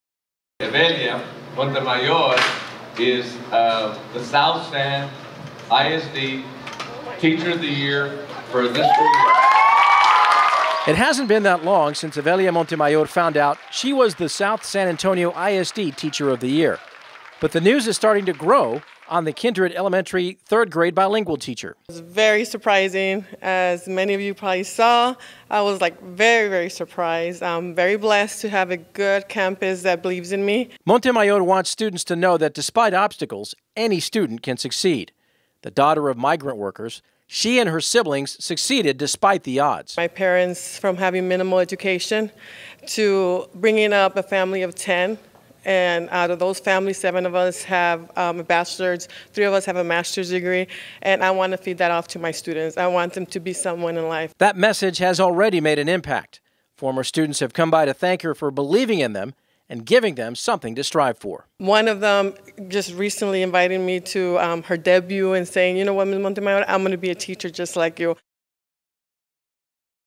Kindred ES 2014 - 2015 Teacher Of The Year
Evelia Montemayor epitomizes what happens to a person when you combine dreams with hard work. The child of migrant workers, she pursued an education to leave that life behind and inspire her students at Kindred ES. Evelia Montemayor is the South San Antonio ISD Teacher of the Year.